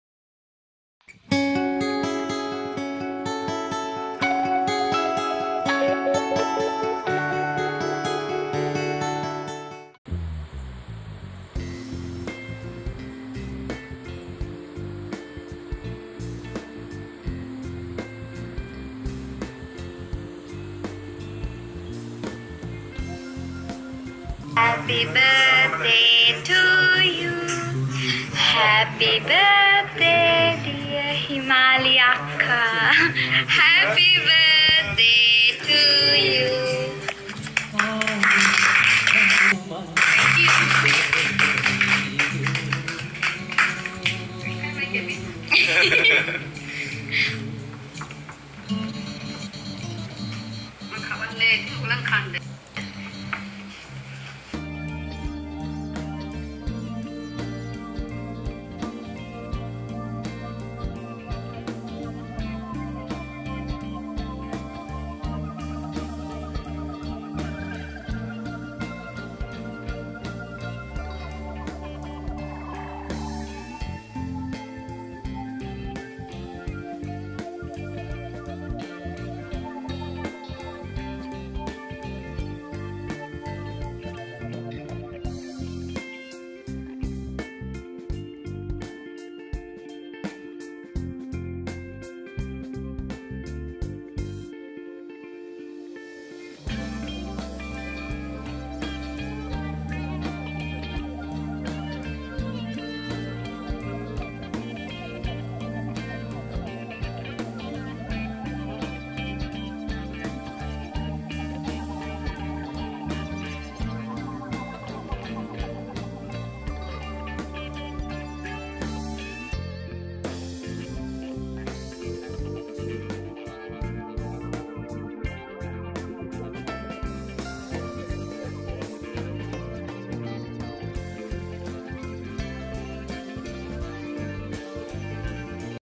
Deweni Inima Udeni Amma Himali Sayurangi Celebrating Birthday With Family Friends HBD Video ❤️🎂❤️
Hi guys!